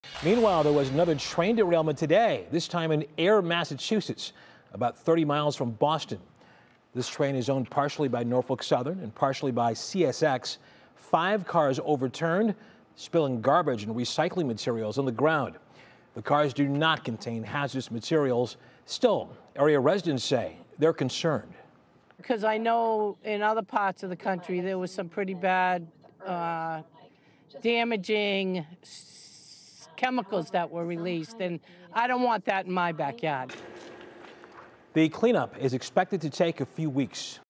Massachusetts train derails; no hazardous cargo reported
The freight train was operated by CSX, although the line is owned jointly with Norfolk Southern.

Five freight train cars derailed in Massachusetts on Thursday, but no hazardous materials were being hauled and there were no reports of injuries, according to authorities.

Authorities say the freight cars toppled over at about 11:30 a.m. Thursday. They were carrying sealed containers of trash and recycling material. The fire department called railway operator CSX and the utility National Grid to the scene, and officials took precautions to protect a nearby waterway. The train was not moving at the time of the derailment. CSX said there were no effects on the environment, and the cause remains under investigation. CSX said the cars derailed on a line jointly owned with Norfolk Southern.

Norfolk Southern is the operator at the center of a fiery derailment in East Palestine, Ohio, in early February in which abut 50 cars derailed, spilling hazardous materials and forcing evacuations of residents.

The cause of Thursday's derailment remains under investigation, the CSX statement said. --